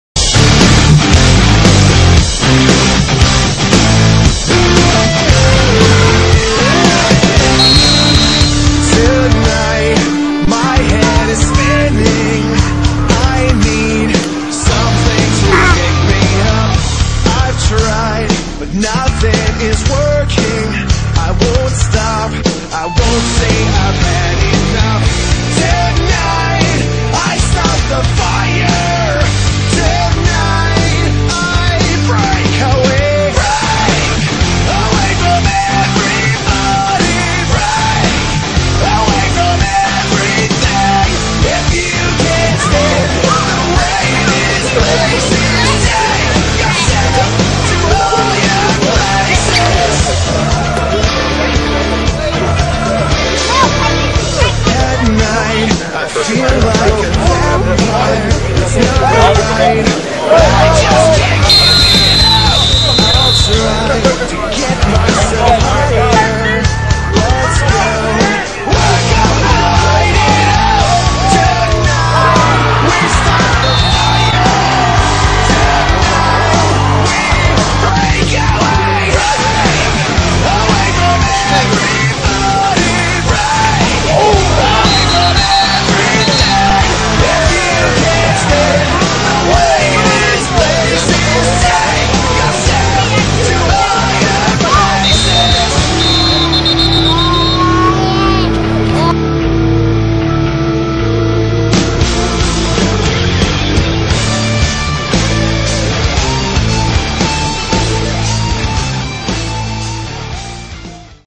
Electric City Chargers Scrimmage @ NEPA All-Stars
Random Footage from the June 25, 2010 Semi-Pro Football Scrimmage.